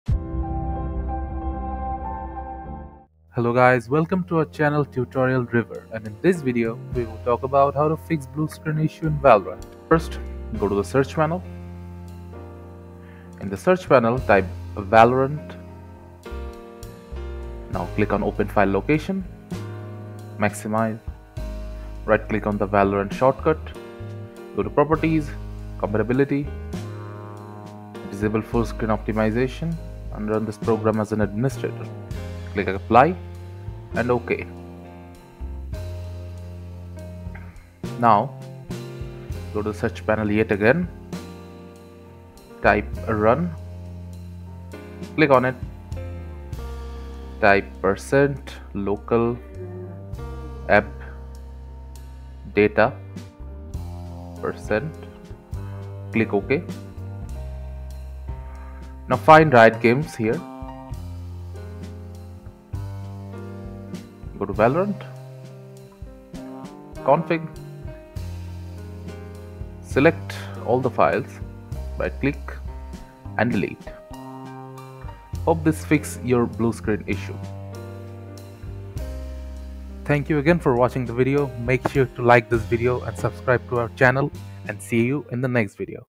How to Fix Blue Screen in Valorant 2026 | 100% Solved
What causes the Vgk.sys Blue Screen? How to fix the Vgk.sys Blue Screen error? For details, you can see this video.

Video Parts:
0:00 Info & Intro
0:11 Go To Search panel
0:24 Valorant Shortcut
1:21 Ending & Outro